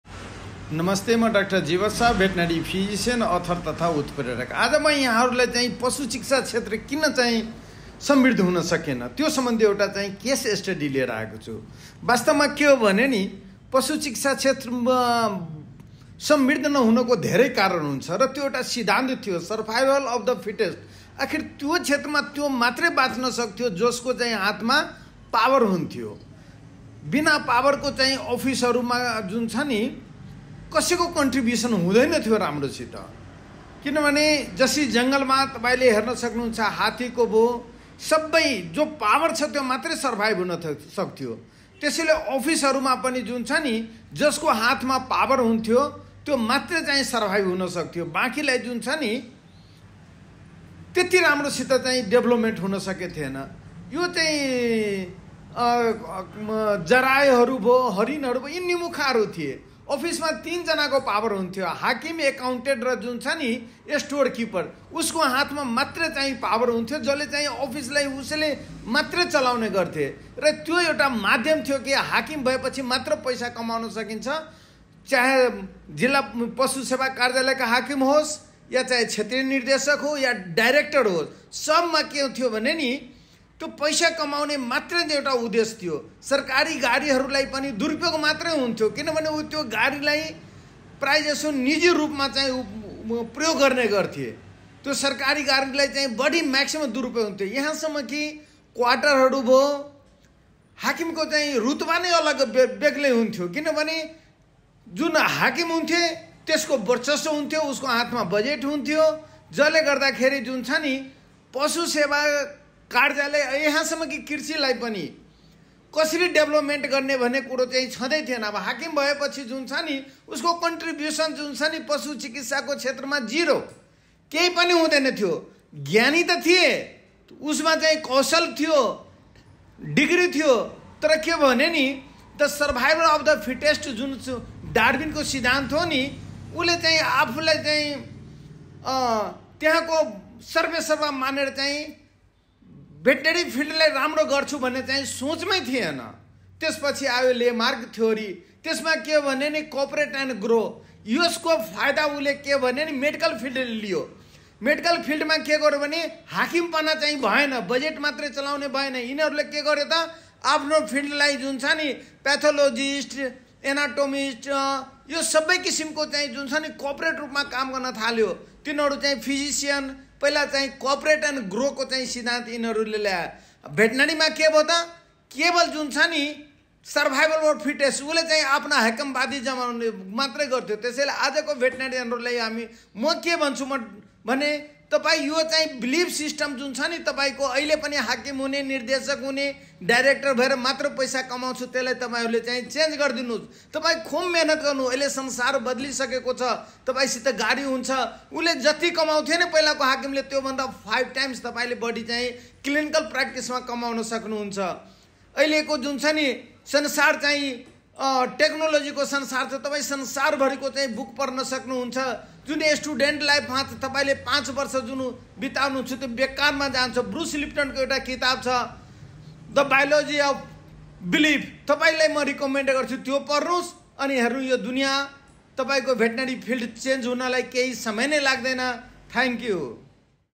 WHY THE VETERINARY FIELD HAS NOT PROSPERED
WHY THE VETERINARY FIELD
HAS NOT PROSPERED ia s motivational video for veterinary students.